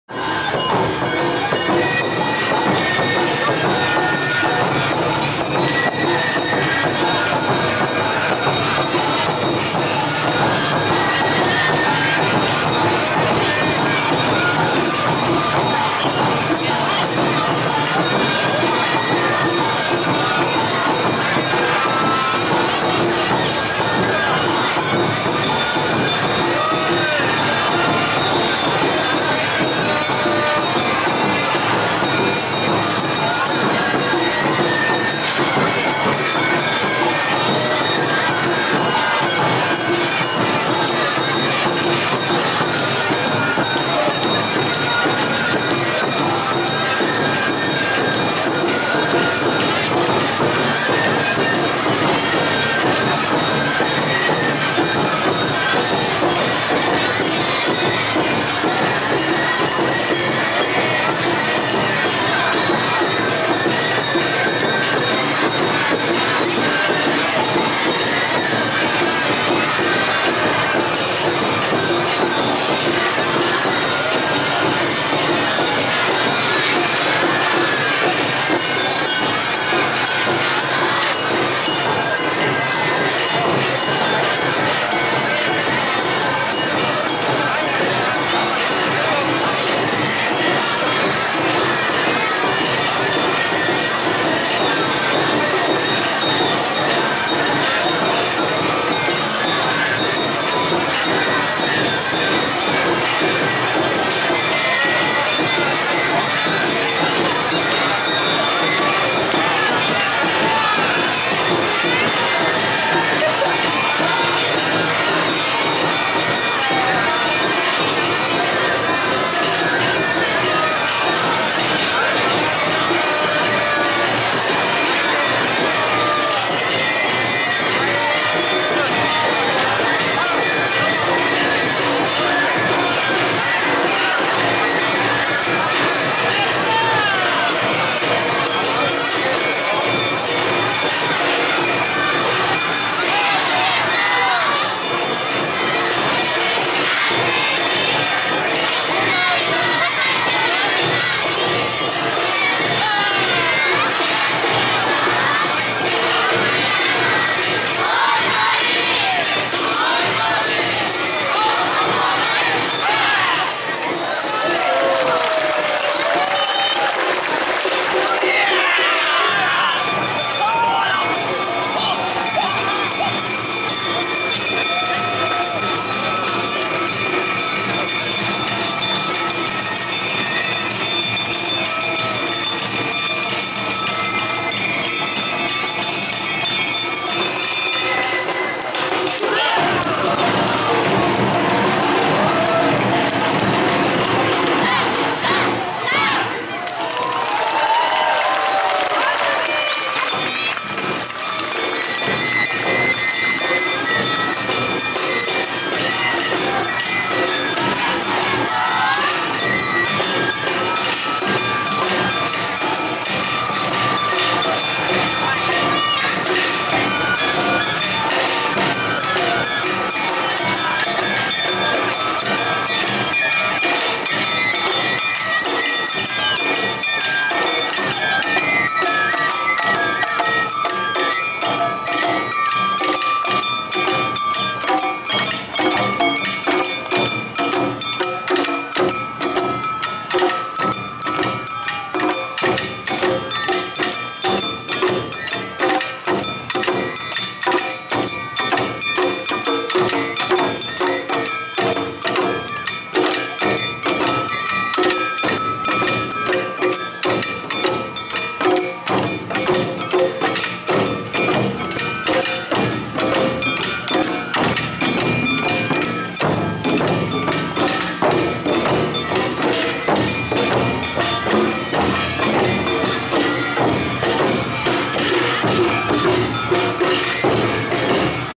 鳳連／第45回 神楽坂まつり 阿波踊り
2016/07/29
第45回神楽坂まつり　阿波踊り2016
【主催】神楽坂まつり実行委員会（神楽坂通り商店会、本多横丁商店会、神楽坂仲通り商店会　共催）
【後援】新宿区